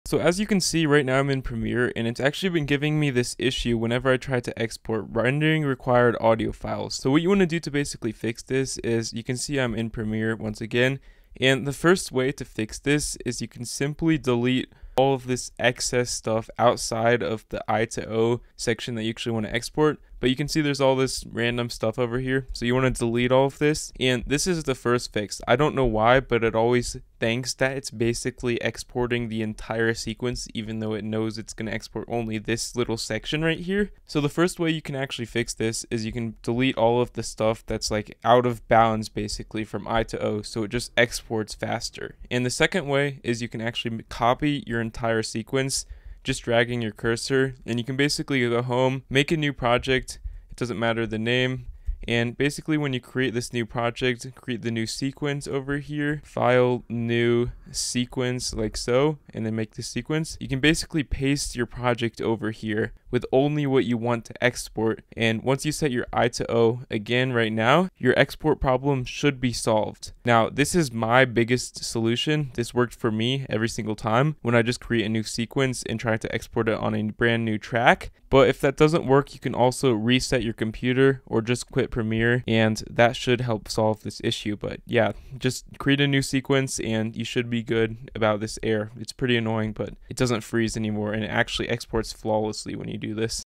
100% FIXED! How To Fix Rendering Required Audio Files Premiere Pro Stuck
0:00 The Issue
0:11 Fix 1
0:42 Fix 2